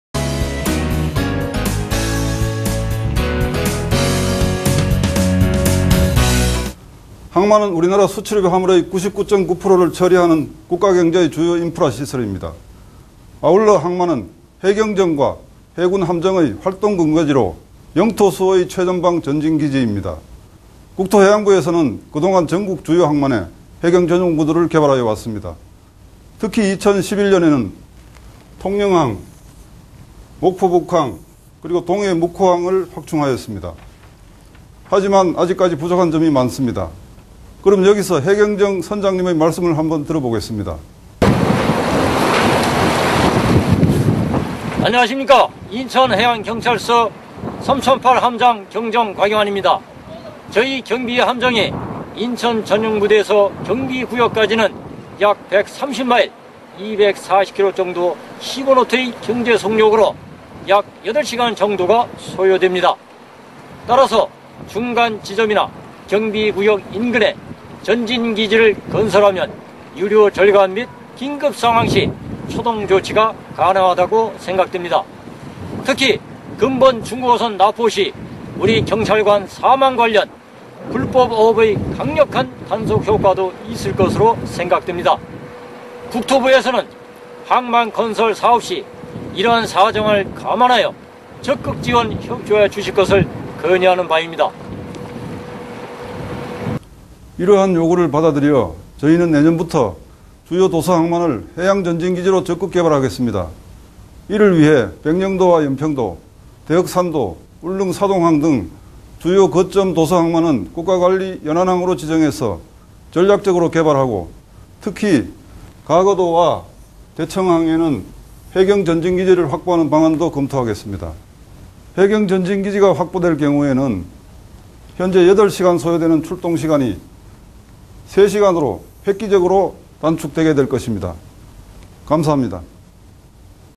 꼼짝마! 불법조업!!
항만은 우리나라 수출입 화물의 99.9%를 처리하는 국가경제의 주요 인프라시설입니다.
아울러 항만은 해경정과 해군 함정의 활동 근거지로 영토수호의 최전방 전진기지입니다.
국토해양부에서는 그동안 전국 주요 항만에 해경전용부두를 개발하여 왔습니다.
특히, 2011년에는 통영항 등 세 개 항만에 전용부두를 확충하였습니다.
하지만 아직까지 부족한 점이 많습니다.
이러한 요구를 받아들여 저희는 내년부터 주요 도서항만을 해양 전진기지로 적극 개발하겠습니다.
이를 위해 백령도와 연평도, 대흑산도, 울릉 사동항 등의 중요한 거점에 위치한 도서항만은 국가관리 연안항으로 지정해서 전략적으로 개발하고, 가거도와 대청도처럼 해안에서 멀리 떨어진 중요 도서에는 해경전진기지를 확보하는 방안도 마련하겠습니다.
해경전진기지가 확보될 경우에는 현재 약 8시간 걸리는 해경정의 출동시간이 3시간으로 획기적으로 단축될 것입니다. 감사합니다.